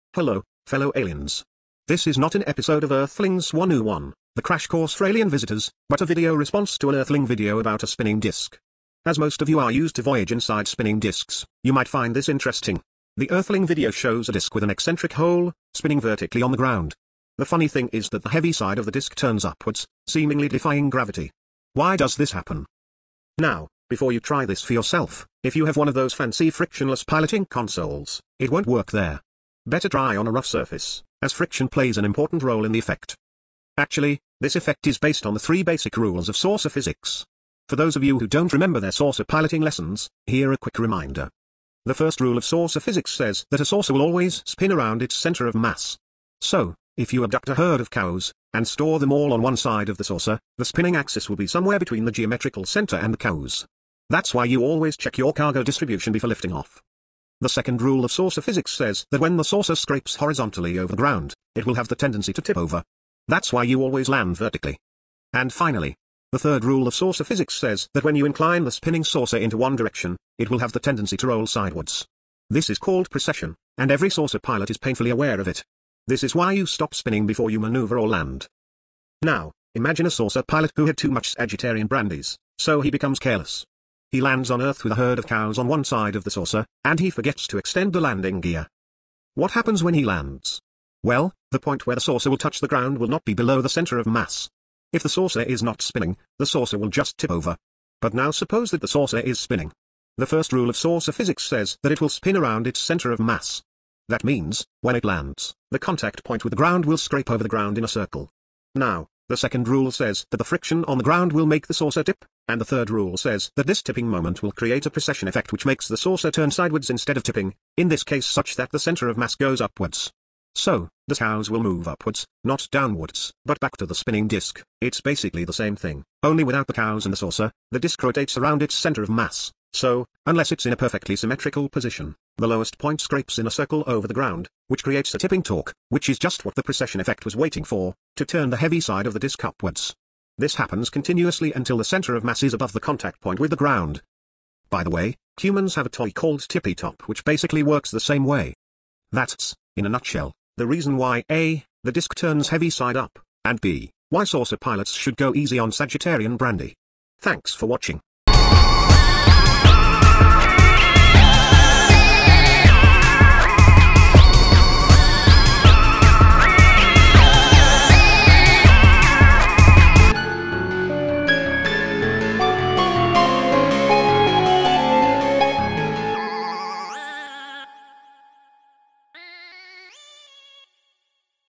Basic Saucer Physics (a video response)
This is not part of the actual course about humans.
an earthling video about the behaviour of a spinning disk. If you are an alien travelling around in a spinning disk, this might interest you.

Credits:
- Concept, artwork and text by Martin Kuppe